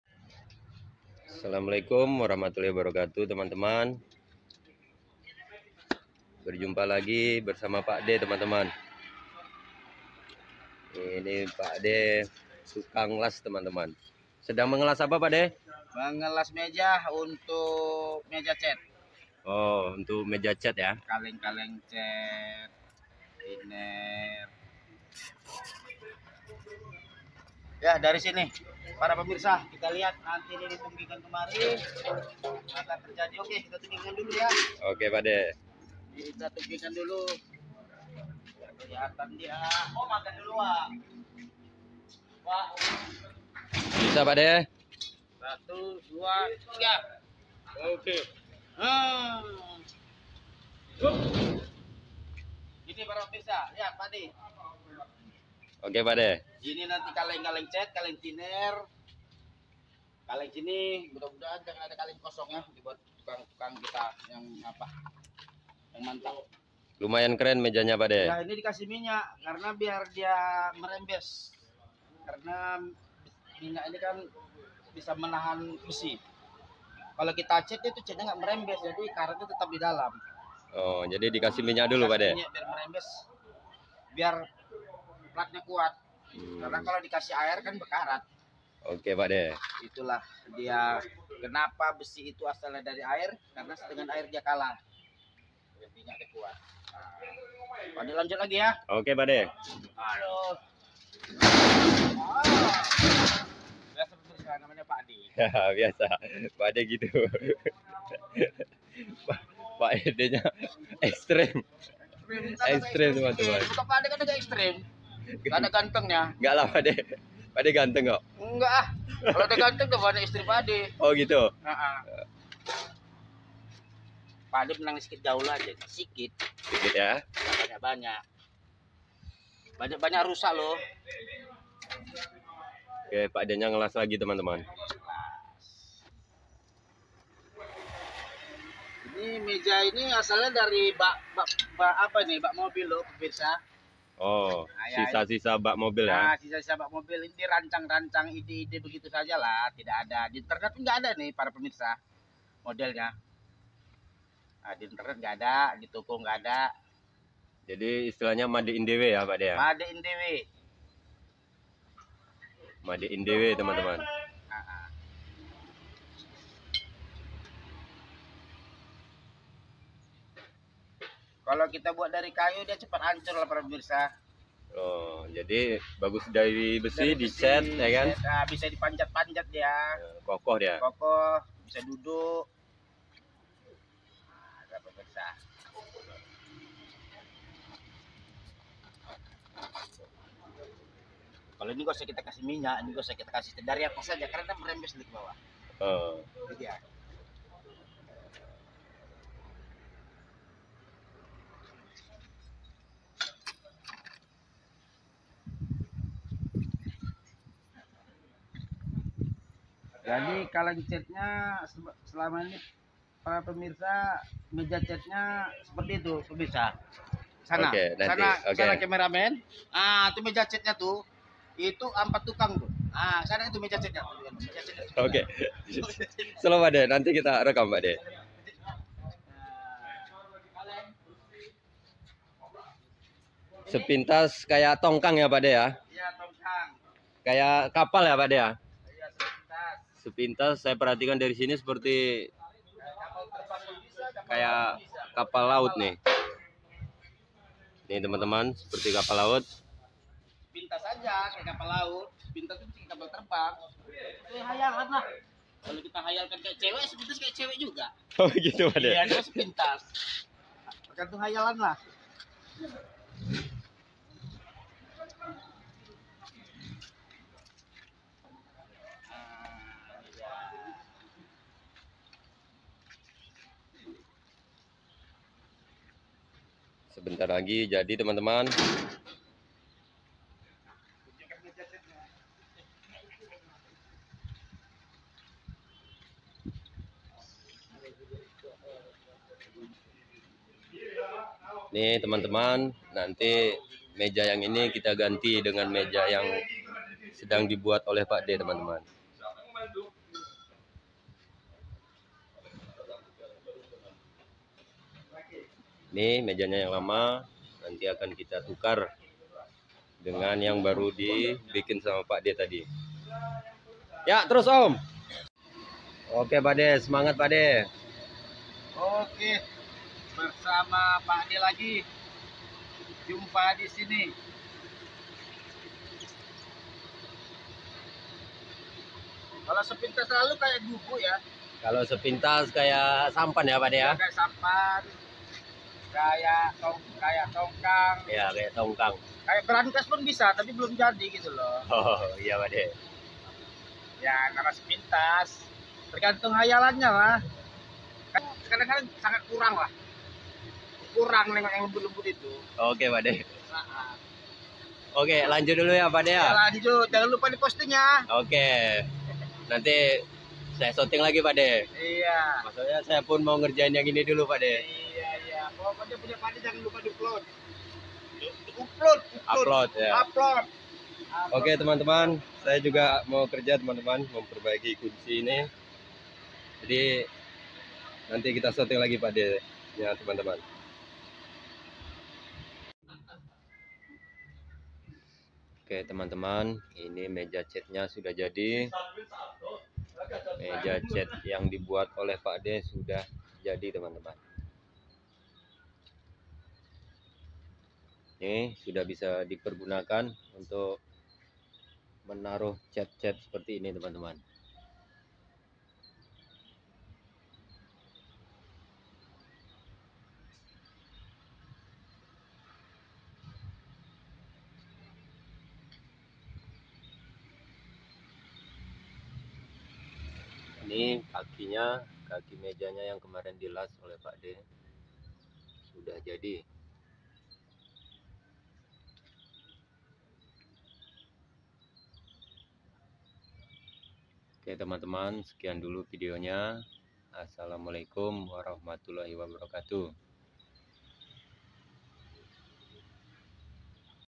Bengkal cat || pak d membuat meja cat.
meja cat dari besi sisa2